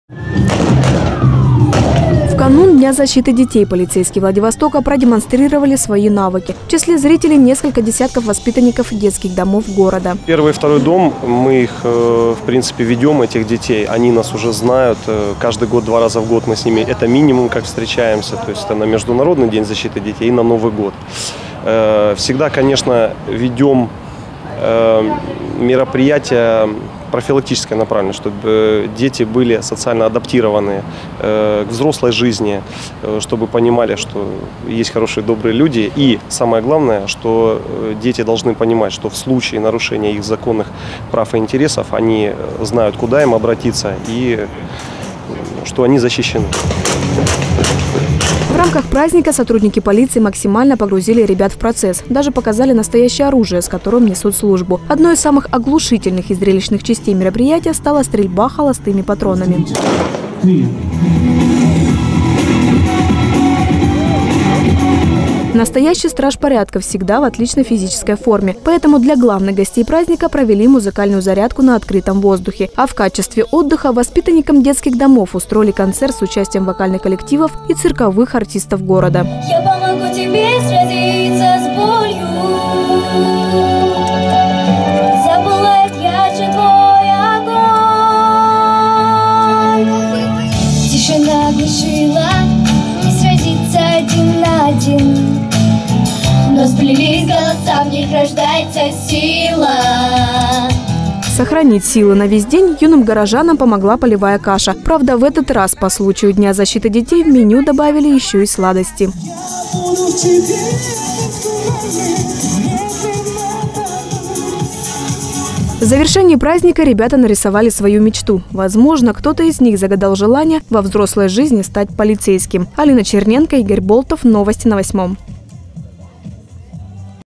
Полицейские научили школьников стрелять во Владивостоке
В канун 1 июня Общественный совет при УМВД России  по городу Владивостоку организовал праздник для воспитанников детских домов №1 и №2. Более 70-ти ребят приехали  на территорию отдельного батальона  патрульно-постовой службы  пострелять из настоящего ружья и попробовать солдатскую кашу.